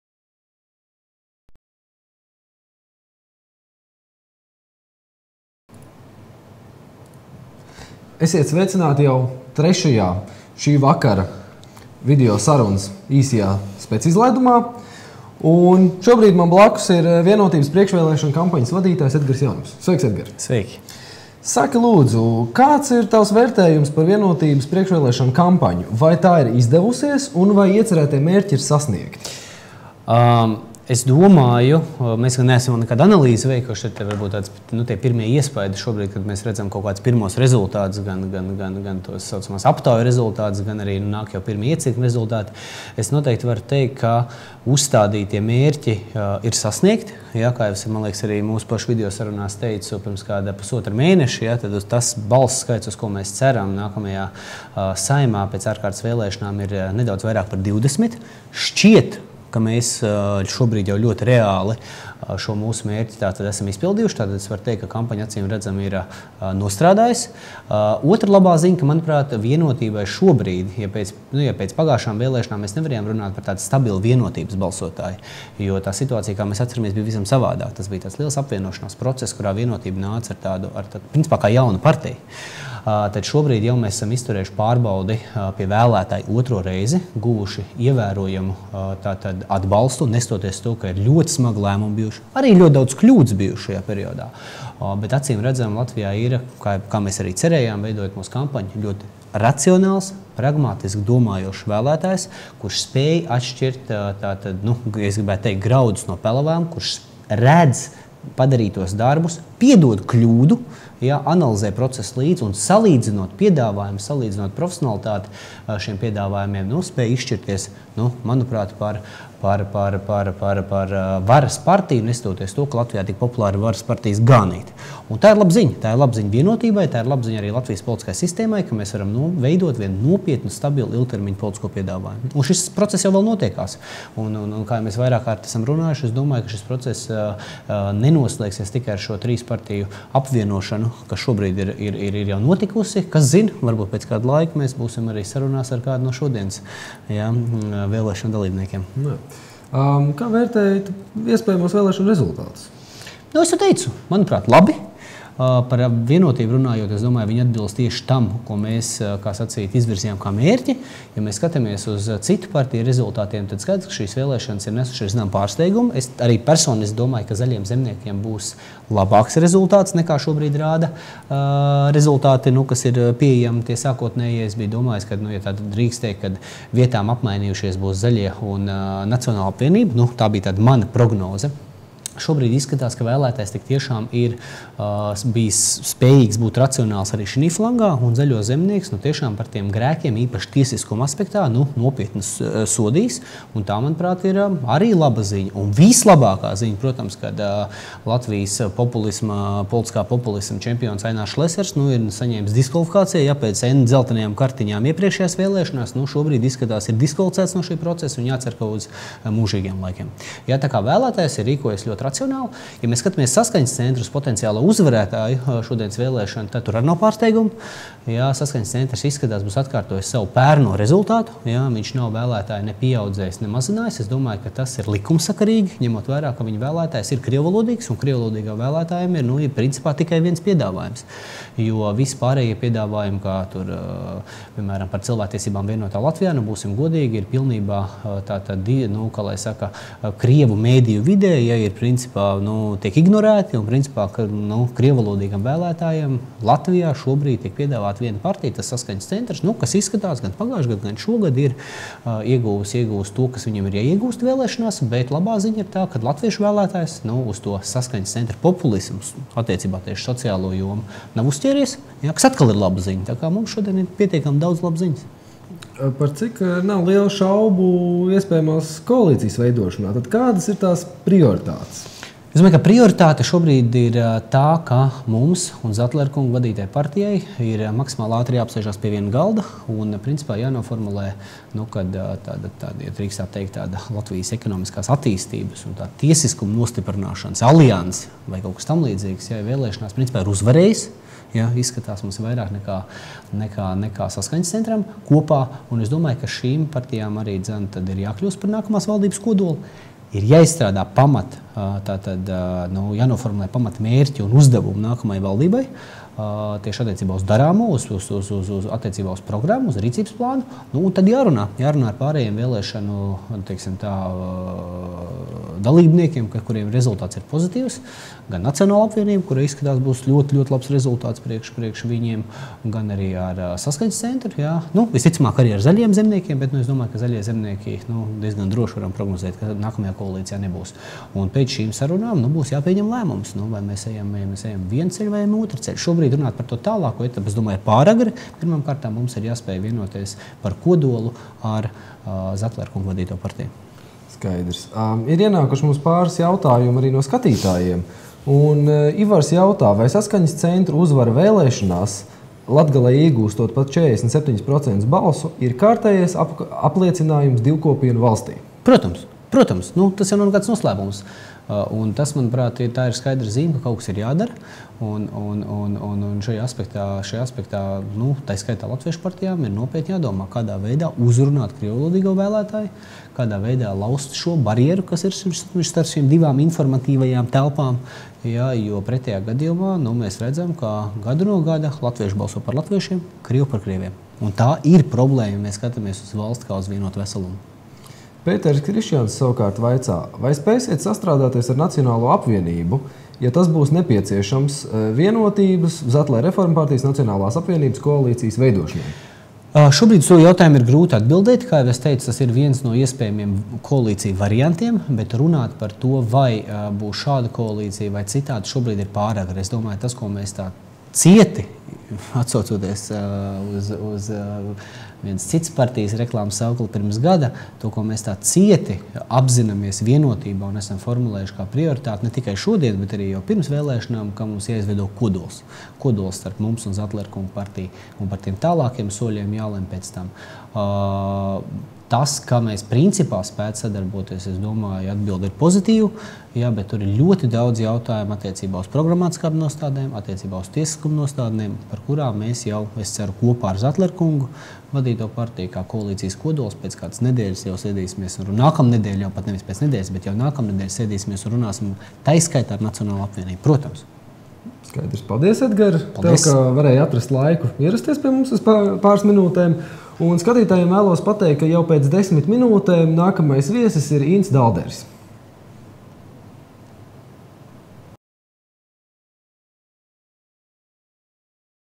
Edgars Jaunups vēlēšanu naktī
VIENOTĪBAS priekšvēlēšanu kampaņas vadītājs Edgars Jaunups vēlēsanu nakts īsajā video sarunā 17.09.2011 - Captured Live on Ustream